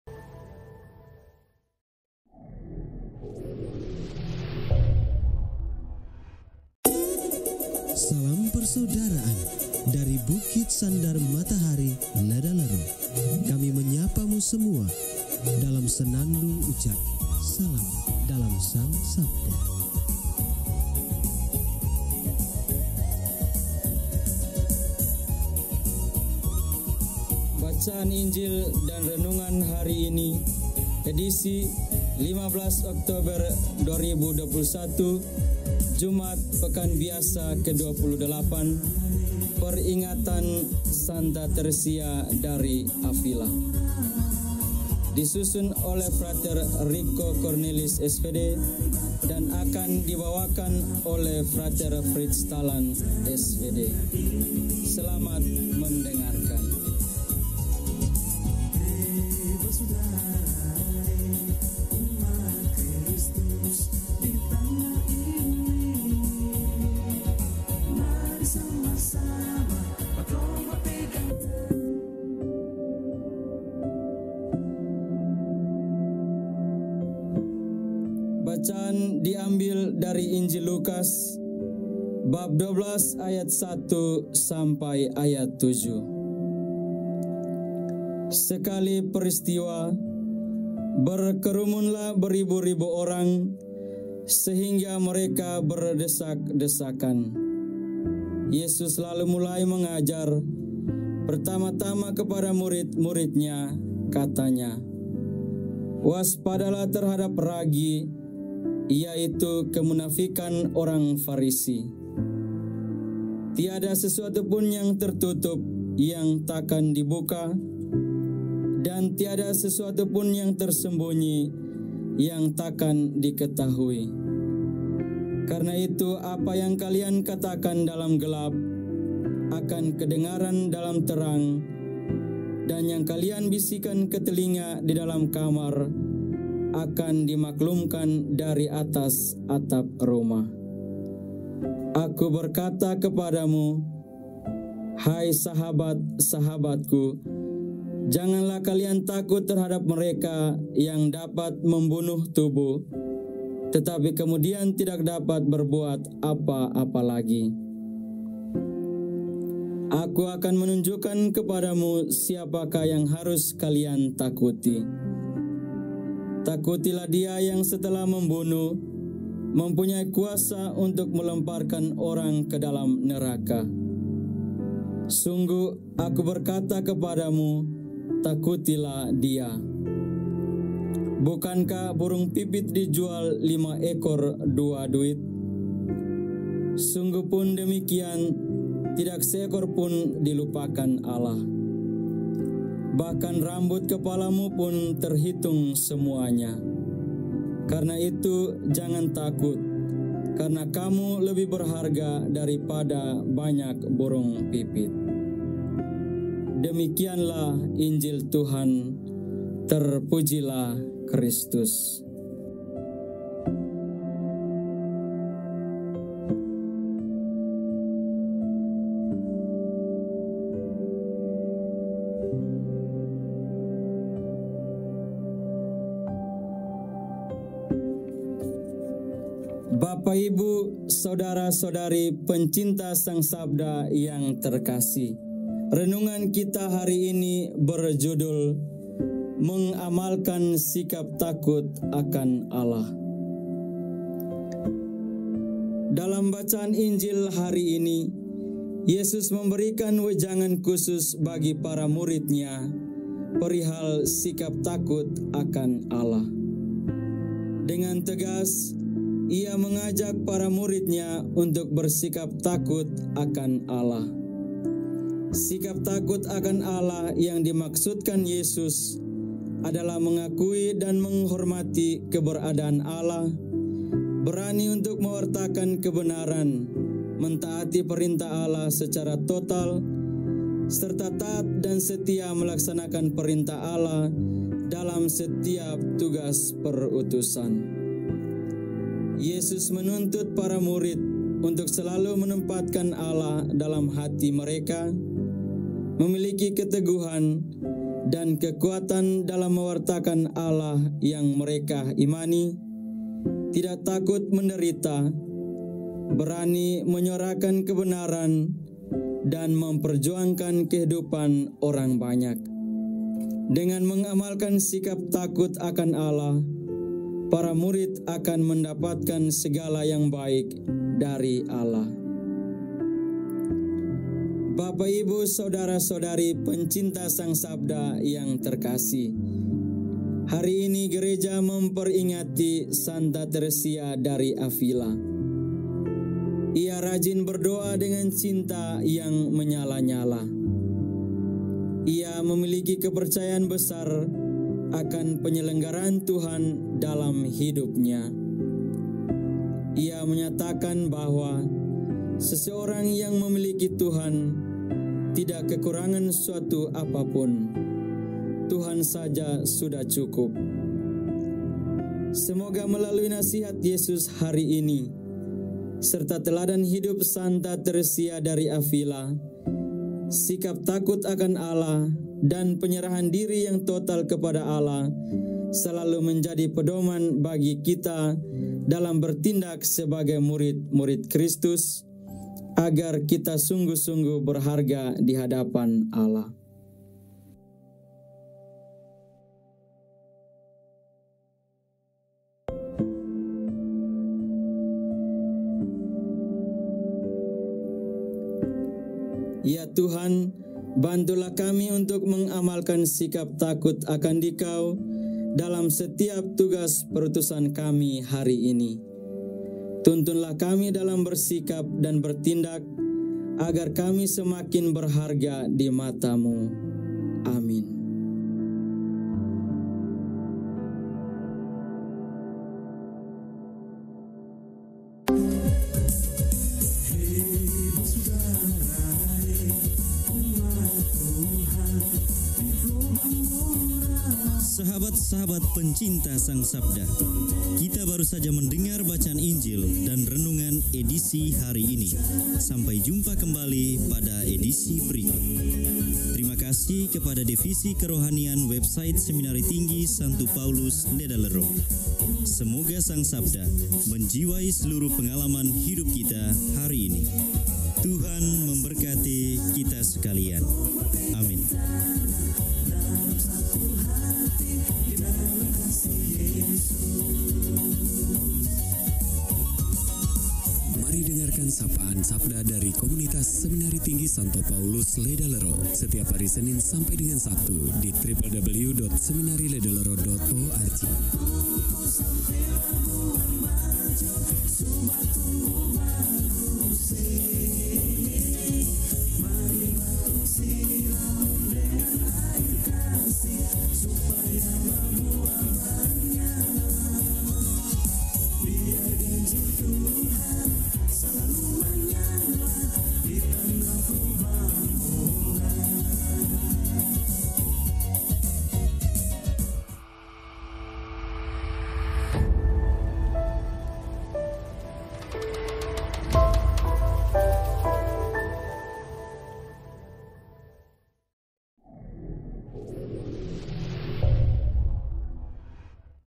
MENGAMALKAN SIKAP TAKUT AKAN ALLAH - (Jumat, 15 Oktober 2021) - Renungan Harian Frater Ledalero
Renungan: Fr. Riko Kornelis, SVD
Pengisi Suara: Fr. Frids Talan, SVD